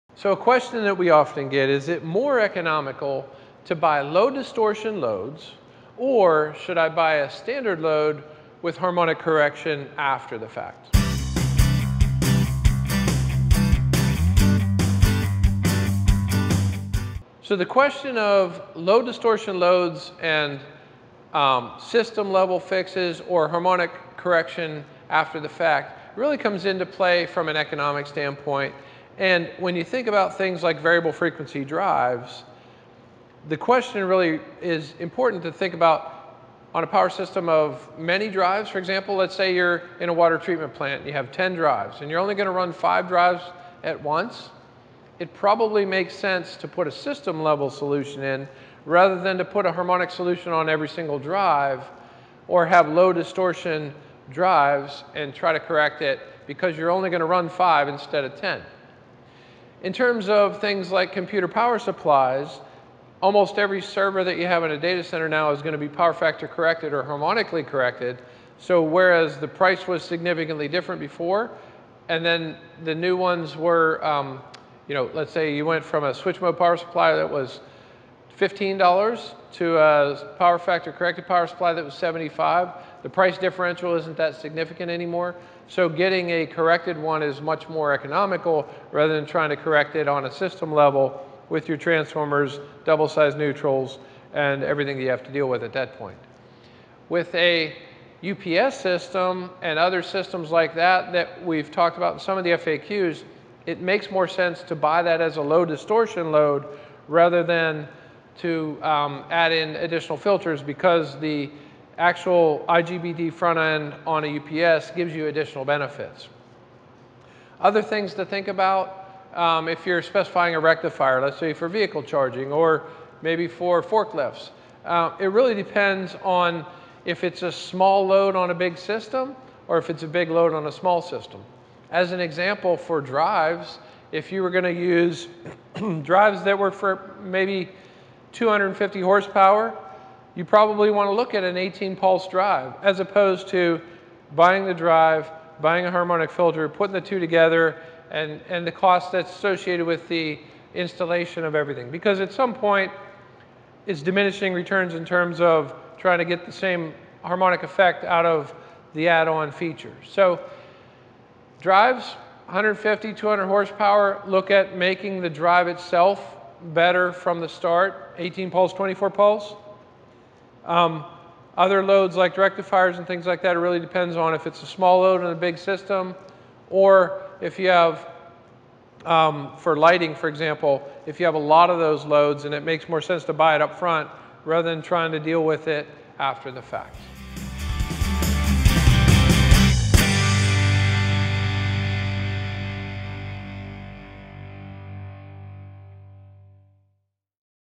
42 - Is it more economical to buy low distortion loads or to apply harmonic correction?
To learn more about Eaton products and our Power Systems Experience Centers

To view more educational videos from Eaton's Power Systems Experience Centers